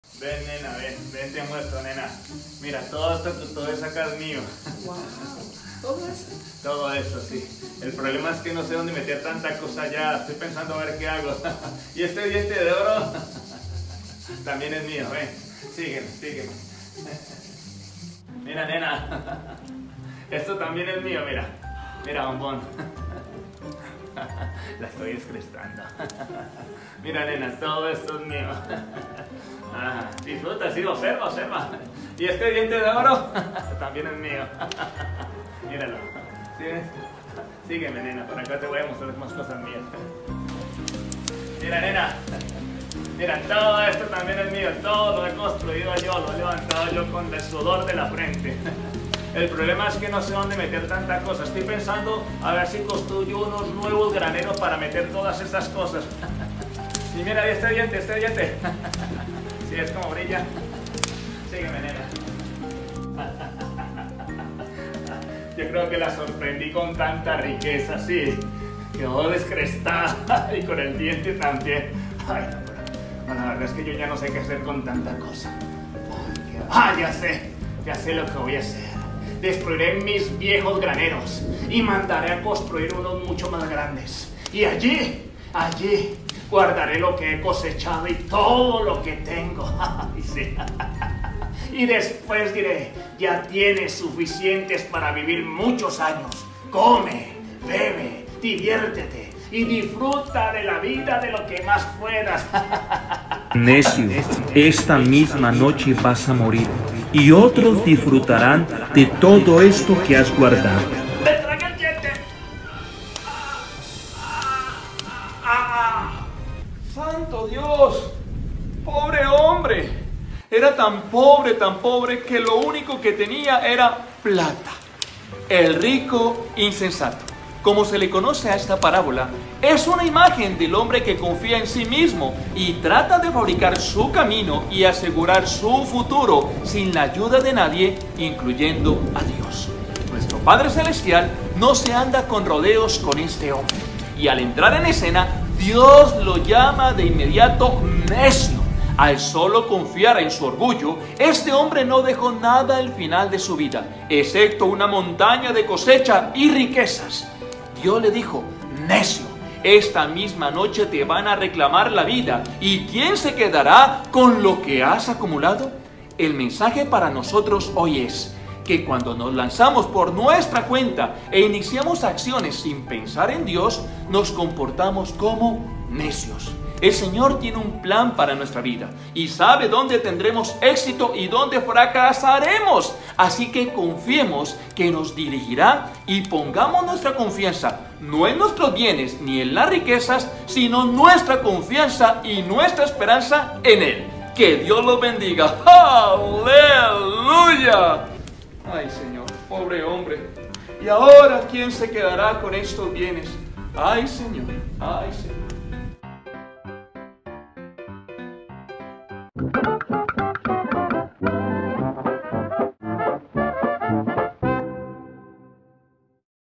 El Rico Insensato
El “rico insensato”, como se le conoce a Esta parábola es una imagen del hombre que confía en sí mismo y trata de fabricar su camino y asegurar su futuro sin la ayuda de nadie, incluyendo a Dios.

Nuestro Padre celestial no se anda con rodeos con este hombre Y Al entrar en LA escena. Dios lo llama de inmediato ¡NECIO !  Al solo confiar en su orgullo, este hombre no dejó nada al final de su vida, excepto una montaña de cosecha y riquezas.